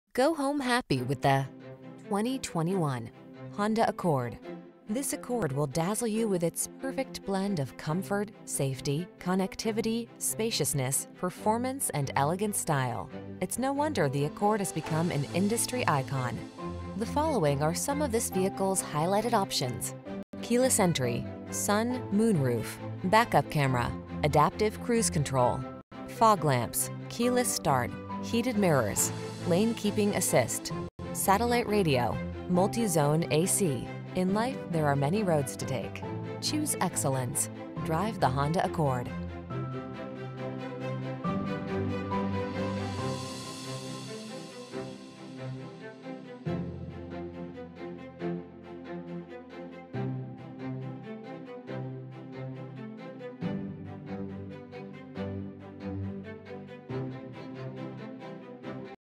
2021 Honda Accord Sedan Marietta, Atlanta, Roswell, Woodstock, Kennesaw, GA 495290
SONIC GREY PEARL New 2021 Honda Accord Sedan available in Marietta, Georgia at Ed Voyles Honda. Servicing the Atlanta, Roswell, Woodstock, and Kennesaw, GA area.
2021 Honda Accord Sedan Sport - Stock#: 495290 - VIN#: 1HGCV2F30MA023856